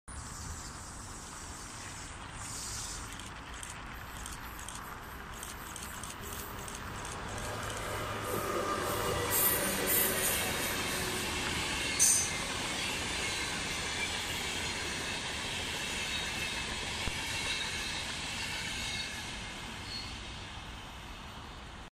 IC 65 saapumassa Kuopioon
IC 65 saapumassa Kuopioon on kuvattu keskiviikkona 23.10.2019.
IC 65 Hki-Ol
Sr1 3011
Vaunut: Edo, Edb, Rx, Eds, Ed, Ed.
Tilaathan kanavani jos et ole tehnyt sitä vielä!